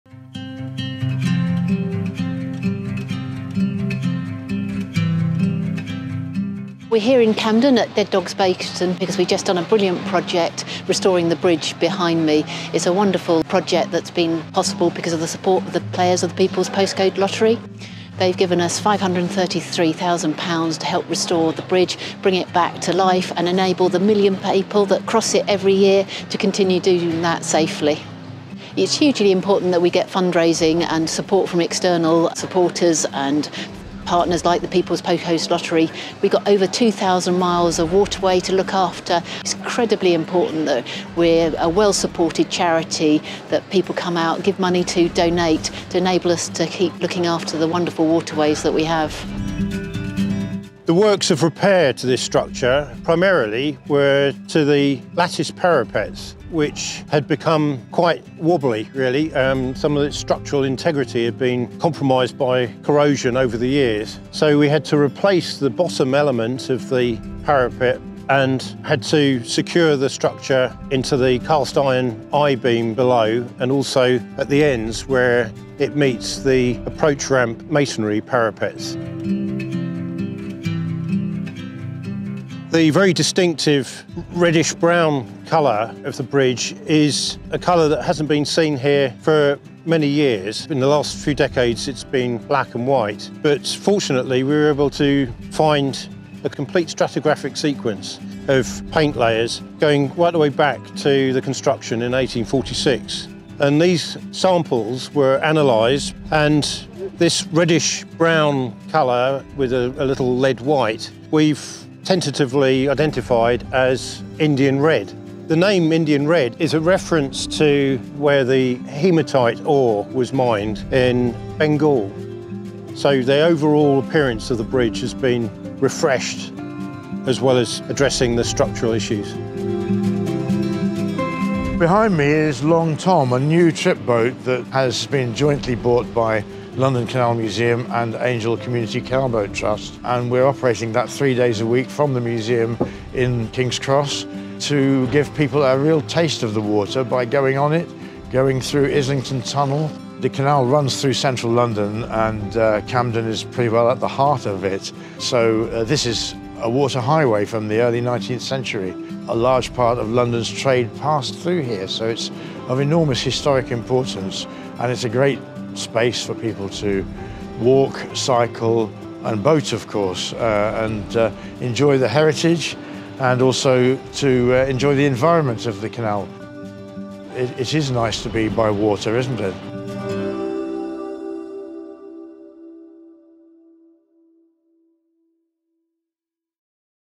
Dead Dog Bridge repair project
We've repaired our busiest canal footbridge in the country - the Grade II Listed Interchange Basin Towpath Bridge on the Regent’s Canal in Camden.